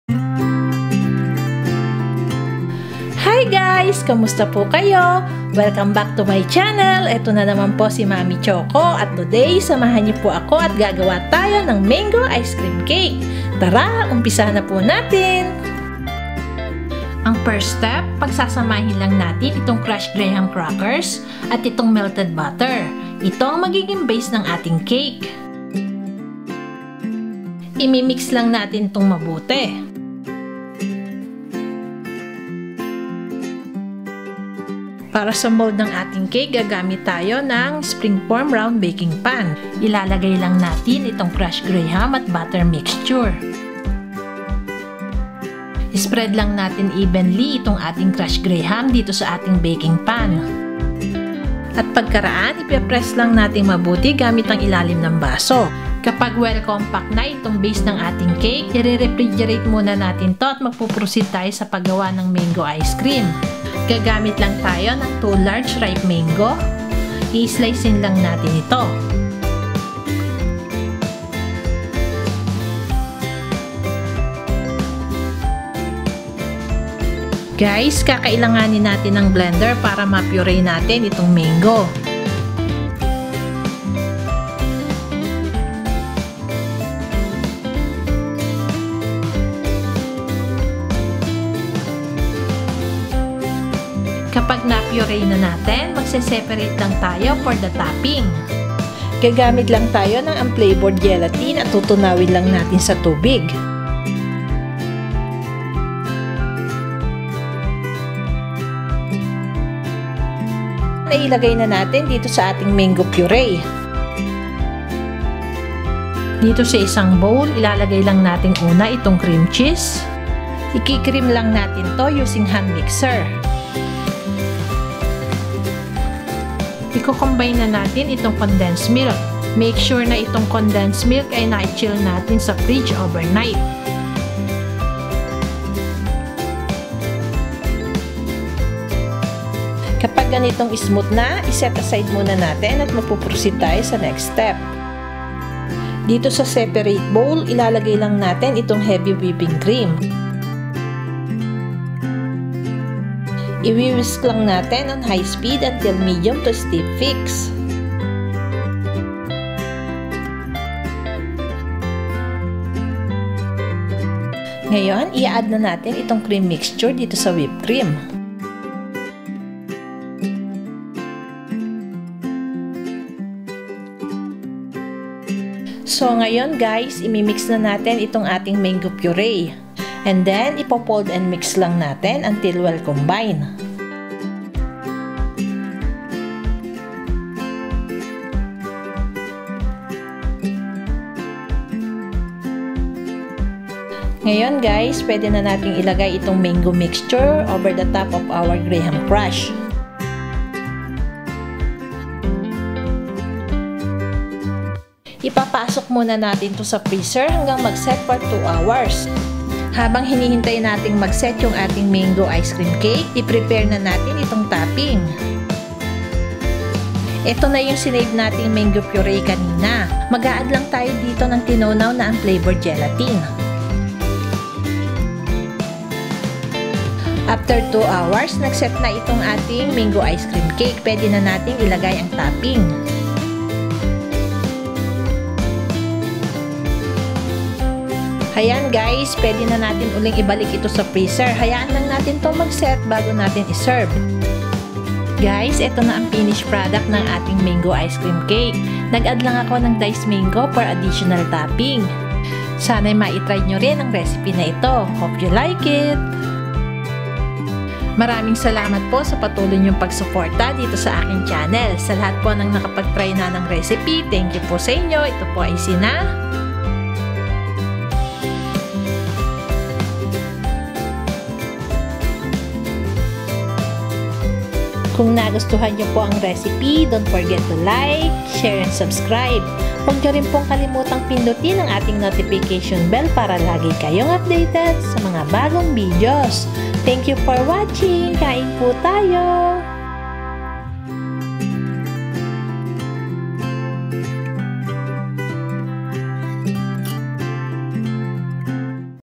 MANGO ICE CREAM CAKE
it's the season of mangoes so treat your family with this gorgeous mango ice cream cake dessert. You have to try this guys just perfect for summer.

INGREDIENTS :

- 2 cups crushed graham crackers
- 6 tablespoons melted butter
- 2 large ripe mangoes
- 3 tablespoons unflavored gelatin powder
- 4 tablespoons water
- 200g cream cheese
- 1/2 cup sweetened condensed milk ( you can add more if the mango you are using is not too sweet.)
-  400ml heavy whipping cream ( chill overnight )

FOR THE TOPPING:
- 6 tablespoons mango puree
- 1 tablespoon unflavored gelatin powder
- 3 tablespoon warm water
-  mango cubes ( optional )

GUYS, I HOPE YOU TRY THIS RECIPE AT HOME!

MARAMING SALAMAT PO SA  PATULOY NYONG PAGSUPORTA DITO SA AKING CHANNEL!

Kain po tayo!